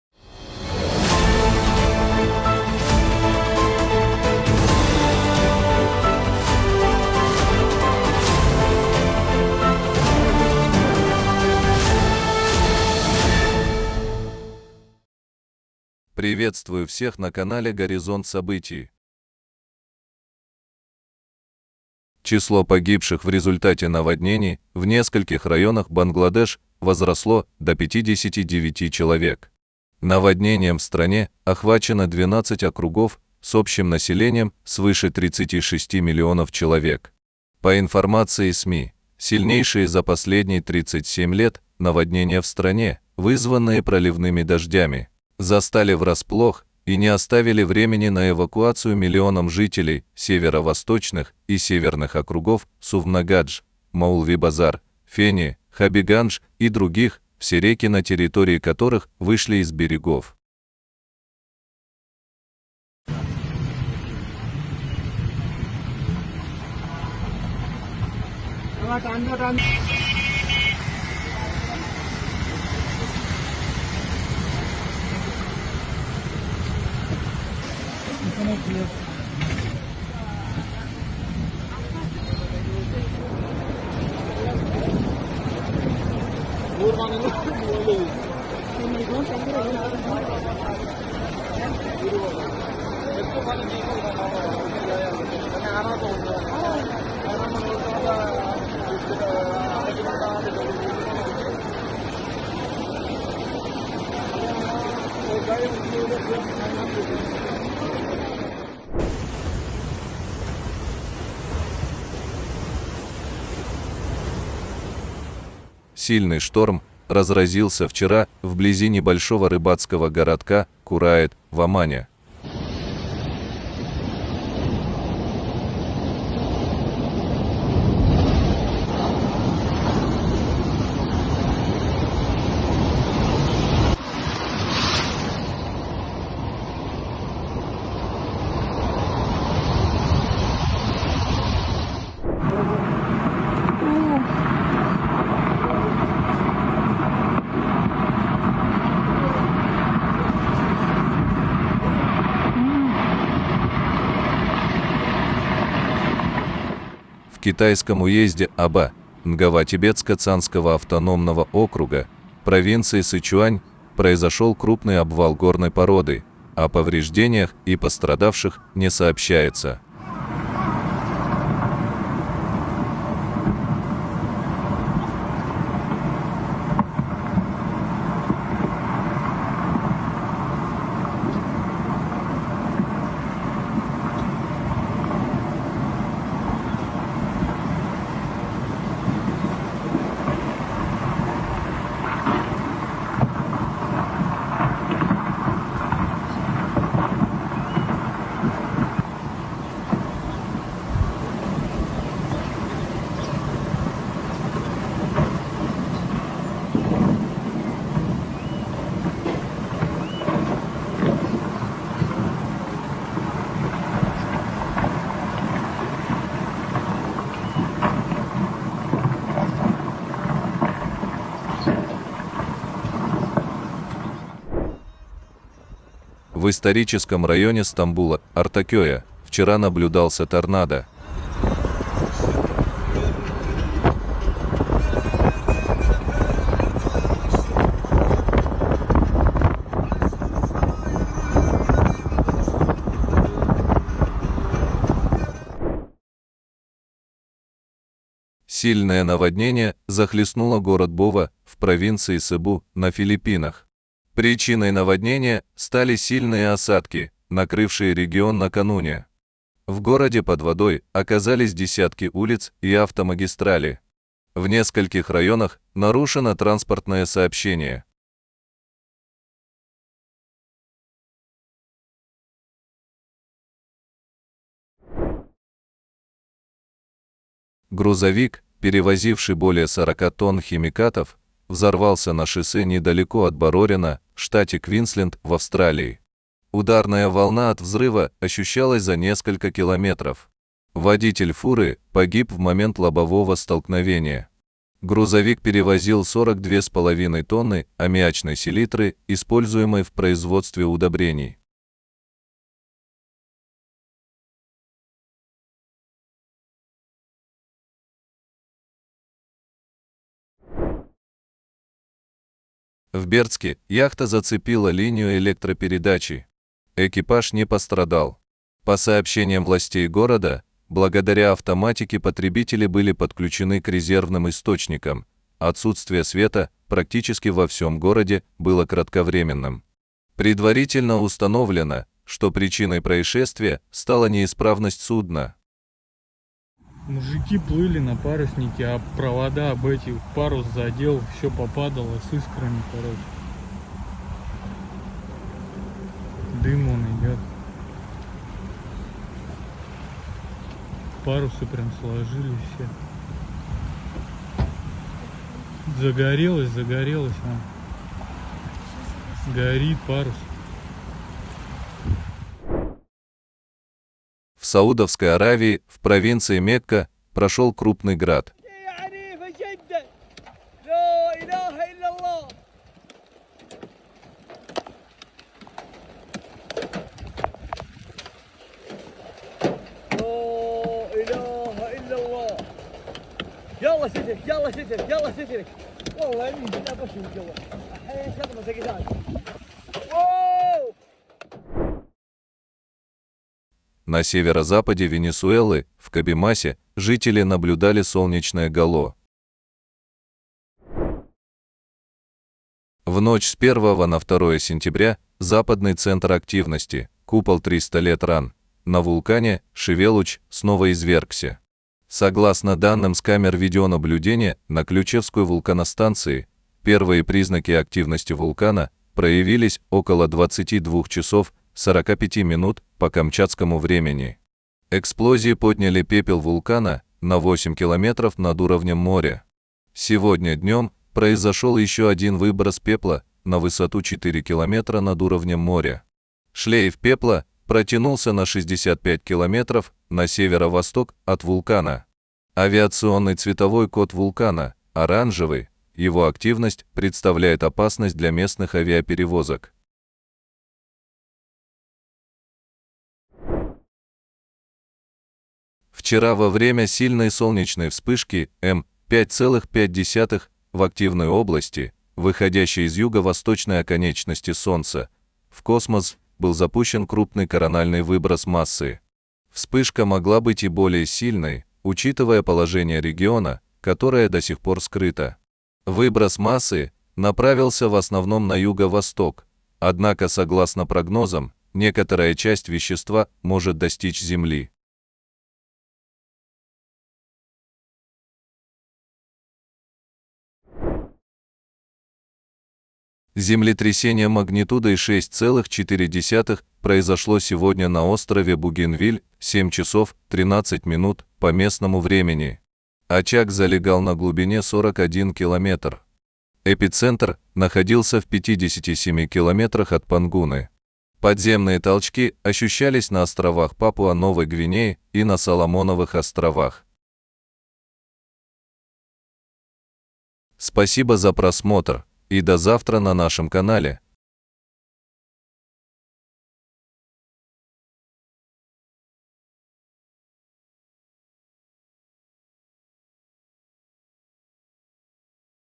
Новости сегодня 02.09.2024. Катаклизмы за день, ЧП, события дня
Катаклизмы сегодня. Новости за день, обзор событий и происшествий на Земле за 2 сентября 2024 года.

00:00 Вступление
00:16 Наводнения в Бангладеш
01:49 Шторм в Омане
02:23 Крупный обвал горной породы в Китае
03:45 Торнадо вблизи Стамбула
04:06 Сильное наводнение на Филиппинах
04:35 В Австралии на шоссе взорвался грузовик с селитрой
05:11 В Бердске яхта зацепила линию электропередачи
06:03 Крупный град прошёл в Саудовской Аравии
06:32 Жители Венесуэлы наблюдали в небе солнечное гало
06:41 Извержение вулкана Шивелуч
07:33 Вспышка М-класса на Солнце
08:11 Землетрясение магнитудой 6.4 в Папуа-Новая Гвинея

СПАСИБО  вам, за просмотр выпуска!  СПАСИБО за внимание ! ДО ЗАВТРА!